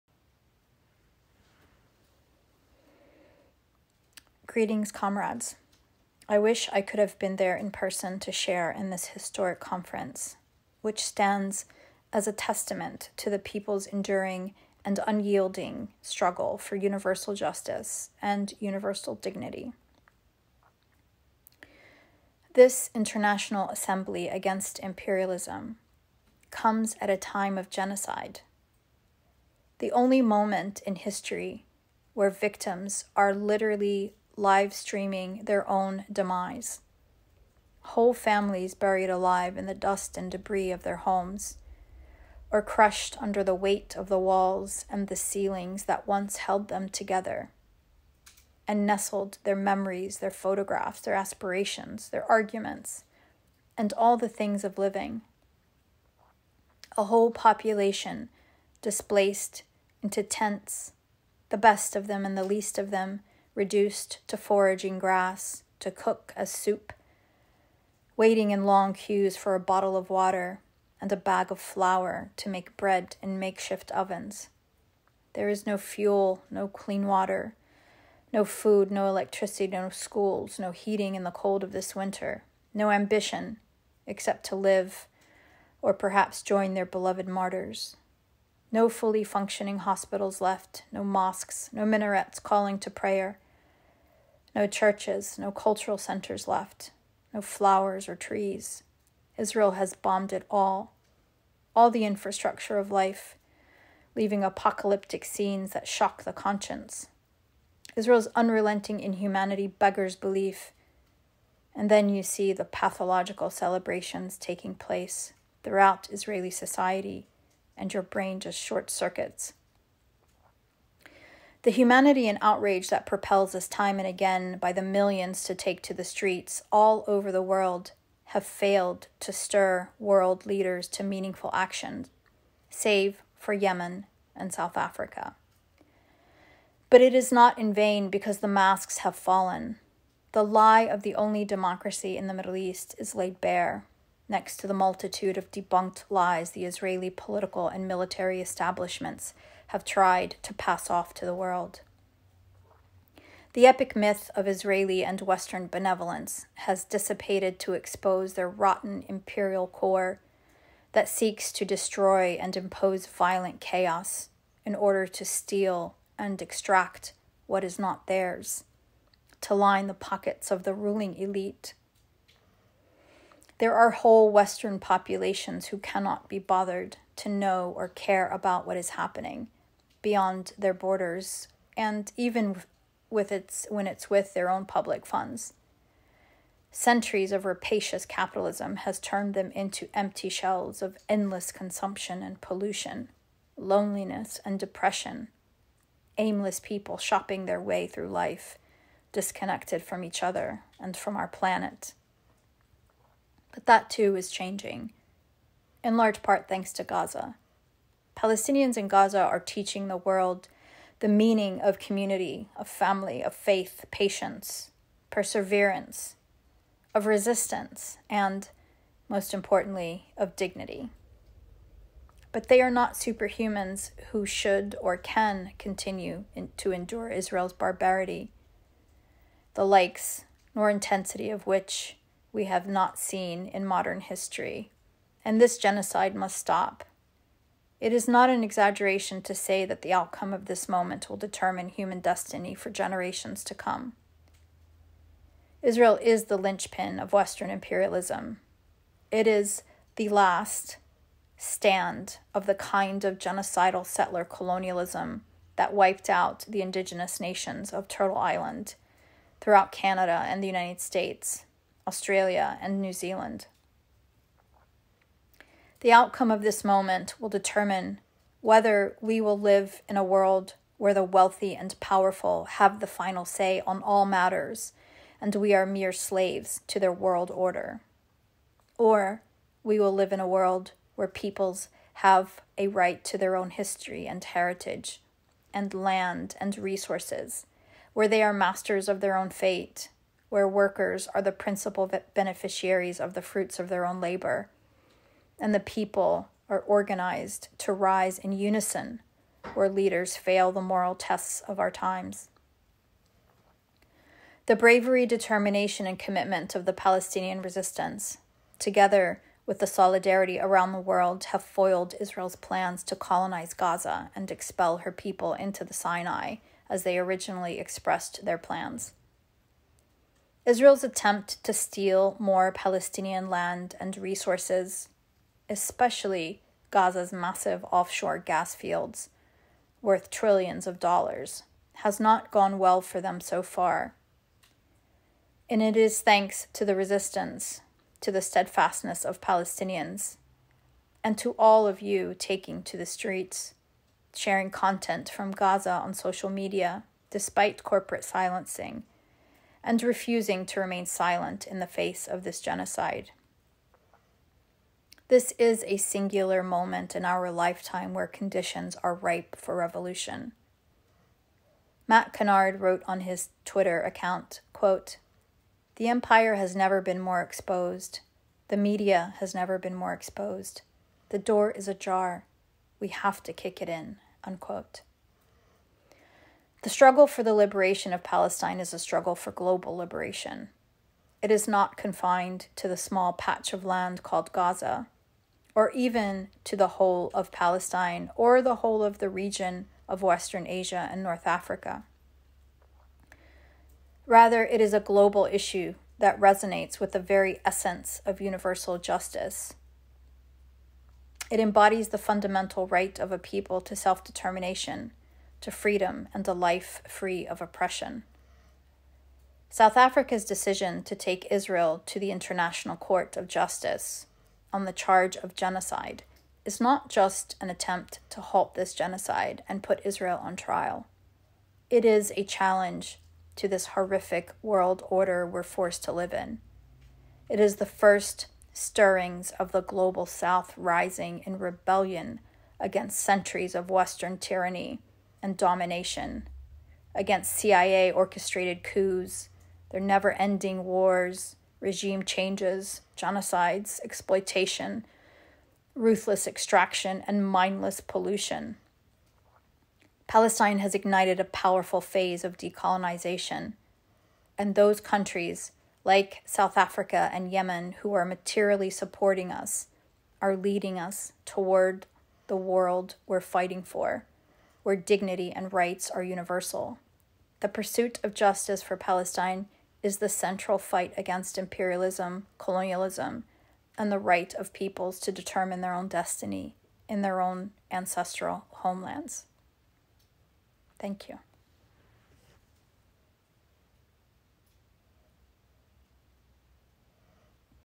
09 Message from Susan Abulhawa
Susan Abulhawa
Prominent Palestinian novelist, activist and member of Workers World Party, Philadelphia branch

Message to the International Assembly Against Imperialism in Solidarity with Palestinian Resistance, Jan. 21, 2024
Here is a report of the event:
Here is a link to the full agenda of over 50 talks and messages
Here are links to each of the 4 panels of the International Assembly
Panel 1